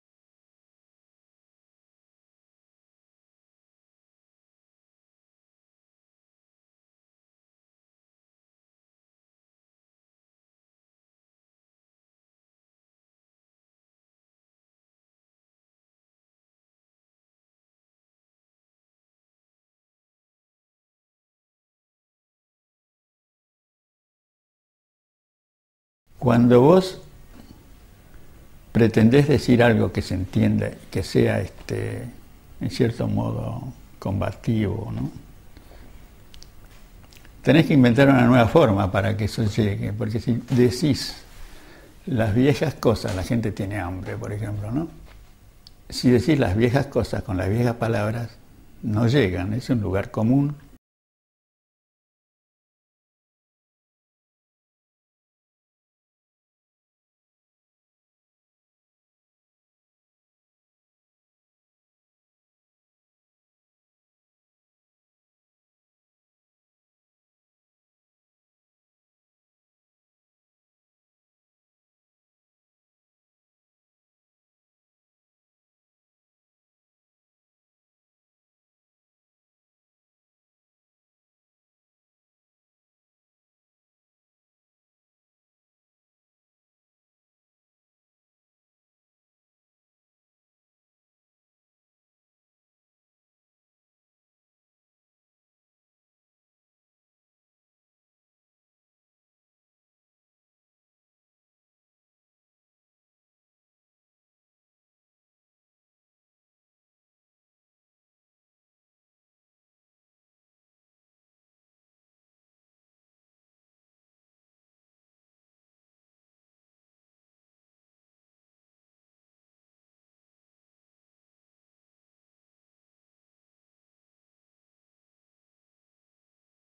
Leon Ferrari
Vida e obra de Leon Ferrari, artista argentino que teve seu trabalho dedicado às causas políticas.